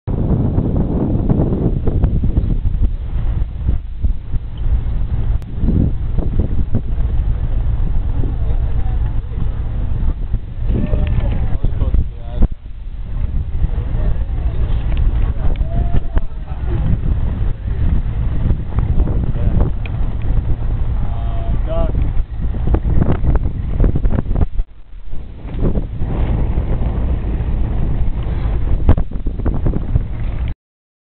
Unfinished Bridge over the Saigon River
river traffic and a bridge under construction near Ho Chi Minh City, Vietnam